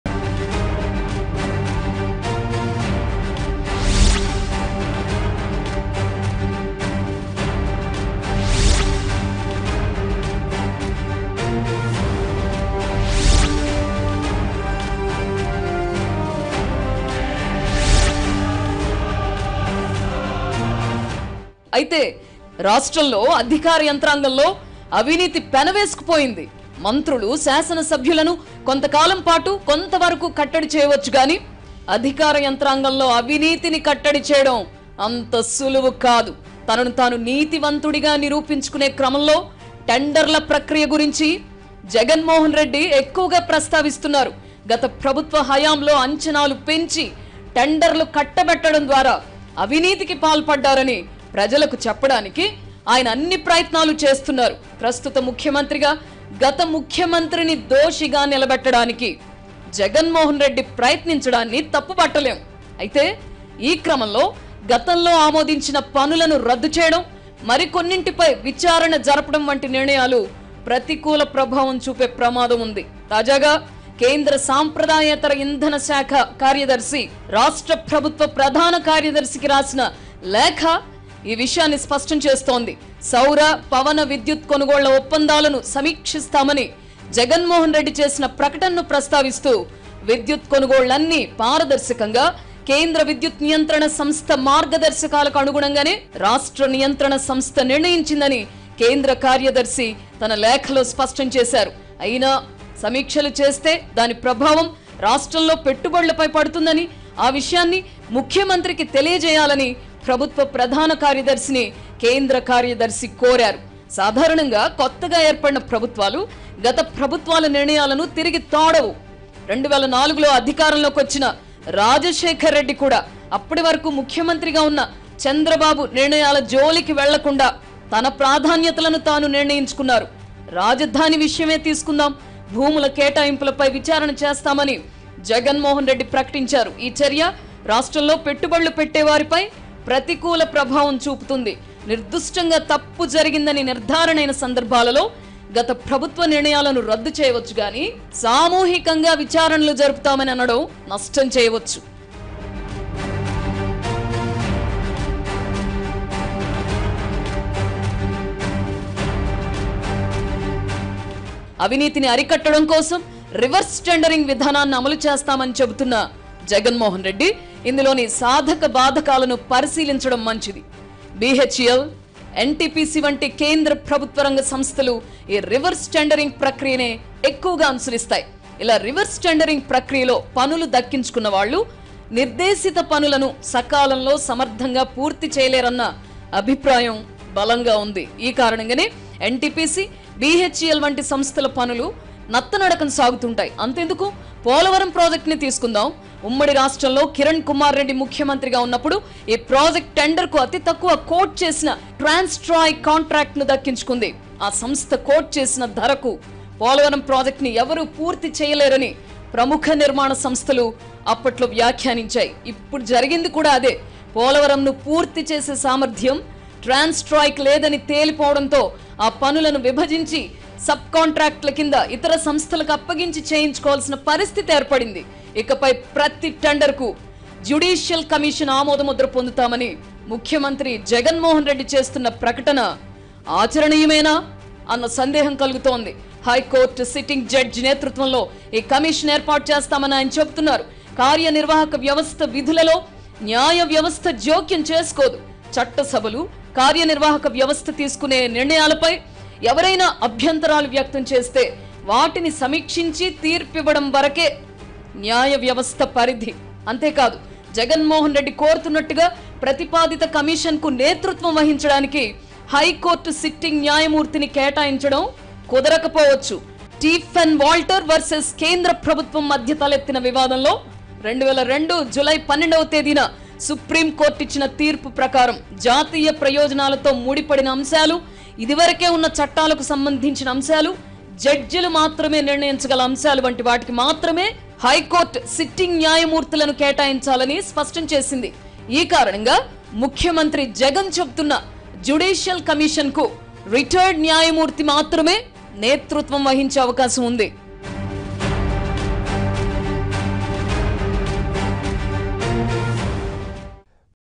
AP CM YS Jagan Efforts To Show Chandrababu As Criminal | Weekend Comment by RK | ABN Telugu
ABN Weekend Comment By RK on AP CM YS Jagan Efforts to show a Chandrababu as a criminal. He stated that the huge corruption took place under the ruling of former CM Chandrababu and he will cleanse every department. While reviewing and stopping the tendered works might impact the investments in AP.